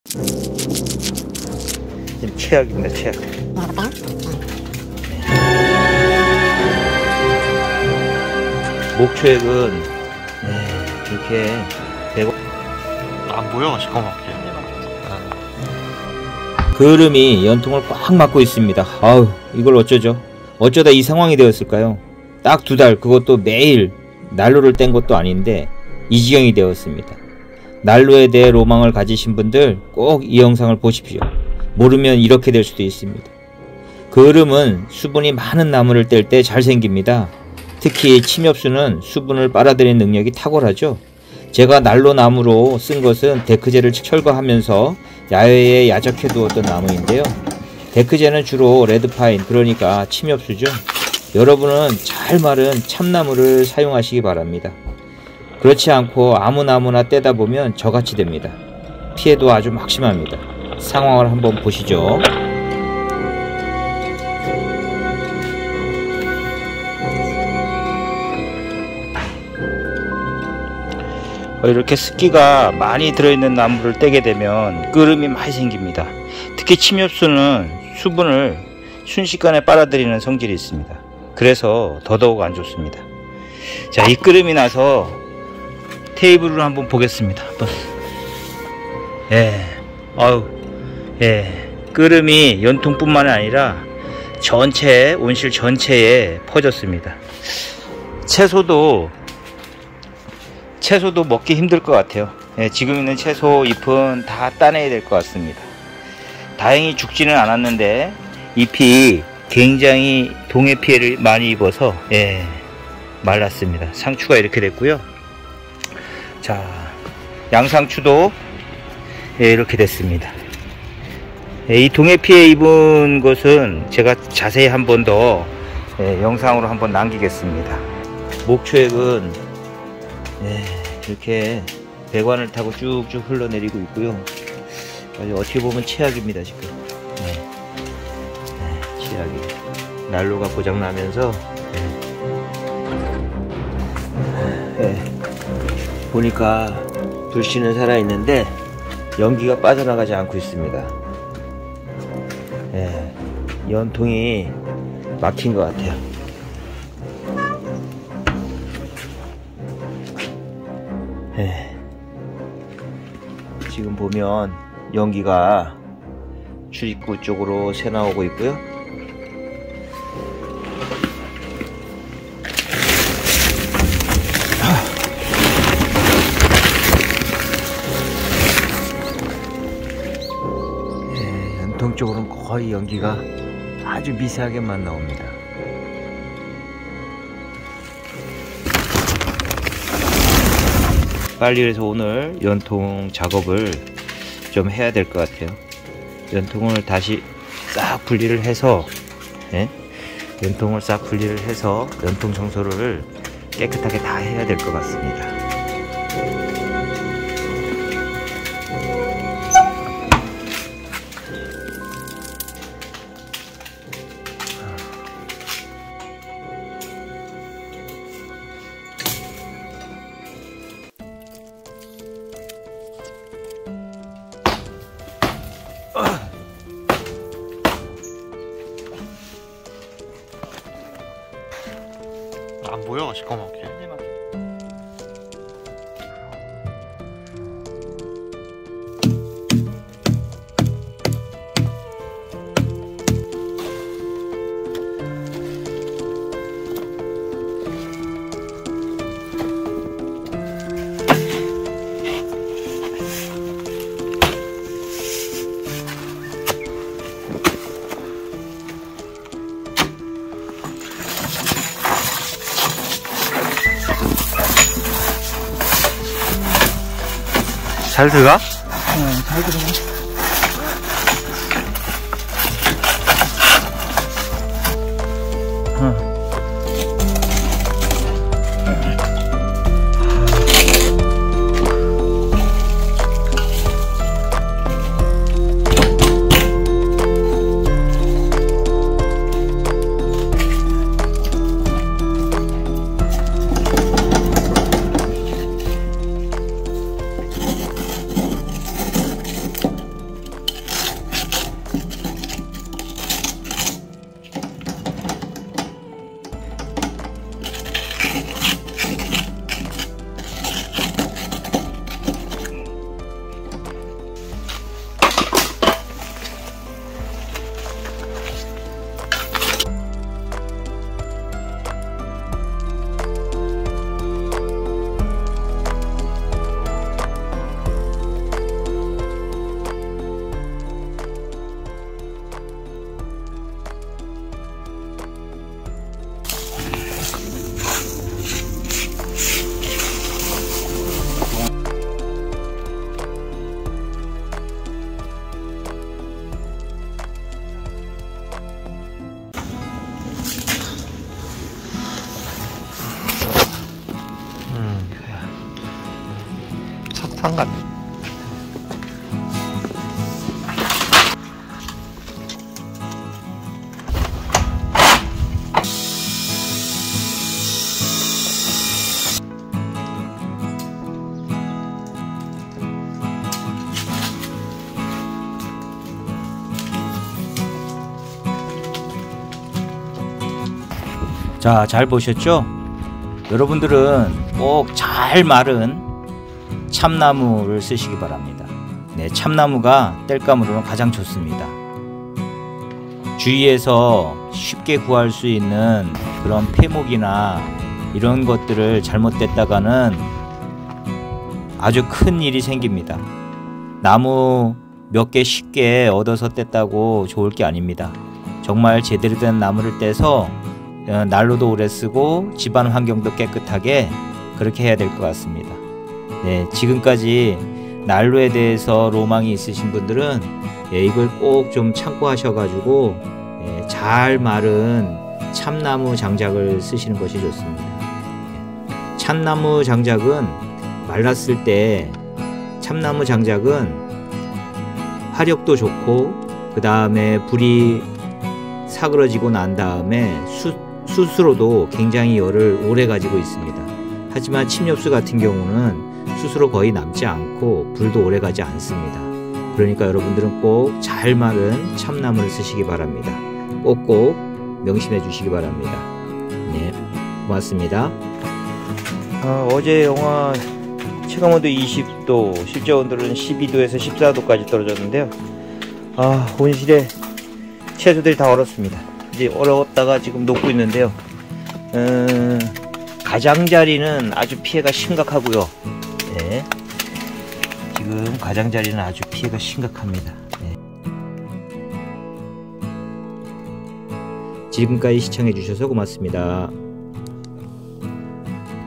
난로 땔감 아무거나 쓰면 안된다
2달동안 사용한 난로 연통이 막히면서 텃밭 온실이 큰 피해를 입었습니다
난로에 땔감을 바르게 사용하는 것이 얼마나 중요한지 알게 되었습니다
난로에 대한 로망이 있으신 분들과 이 영상을 공유하고 싶습니다
이미 알고 계신분들이 많은 것으로 알고 있습니다
좋은 이야기 아래에 남겨 주세요